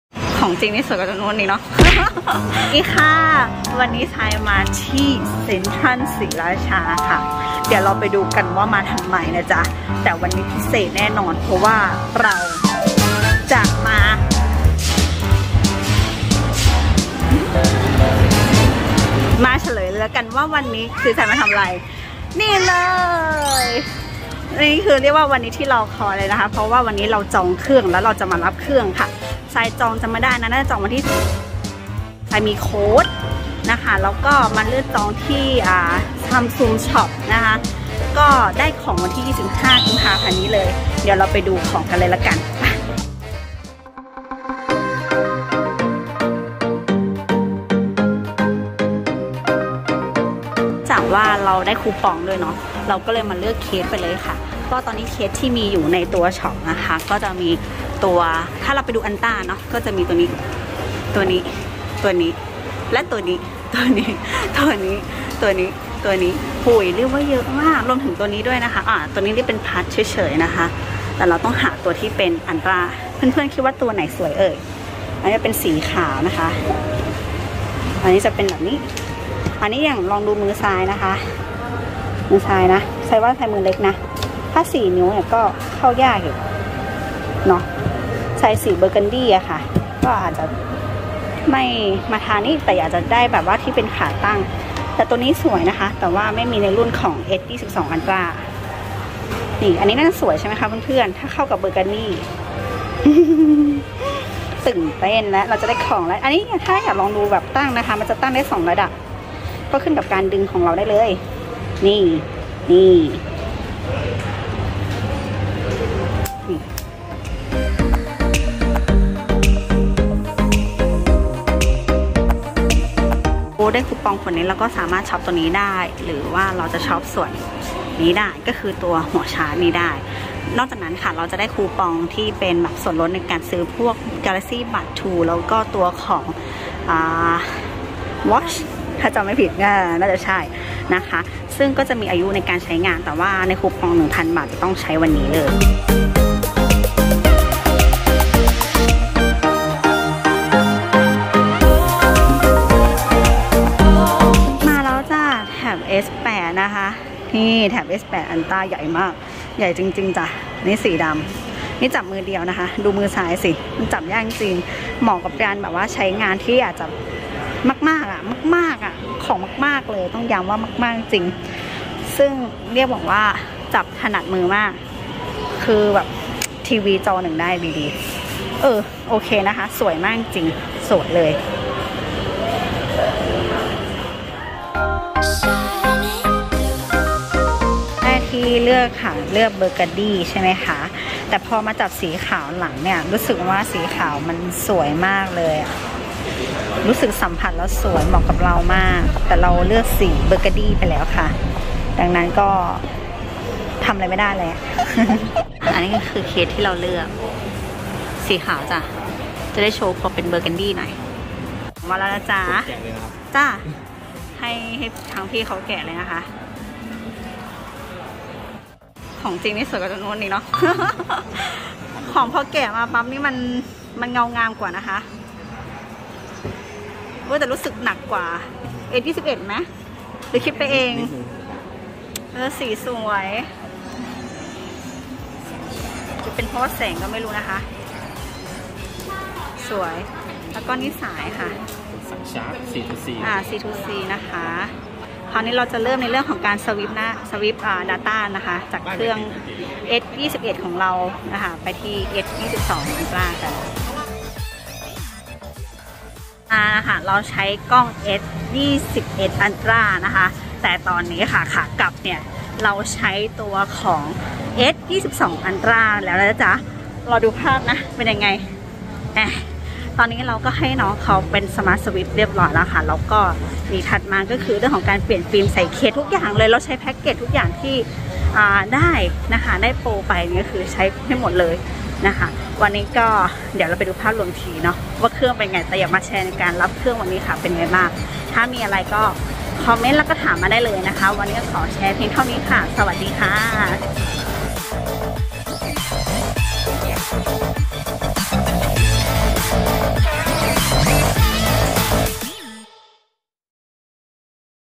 ไปรับ S22 Ultra กัน | Samsung Galaxy S22 Ultra
ไปรับเครื่อง Samsung S22 Ultra ด้วยกัน  วันนี้มาเร็วๆหน่อยนะคะ  เดี๋ยวดึก T^T  เพราะต้องโอนข้อมูลจากเจ้า S21 Ultra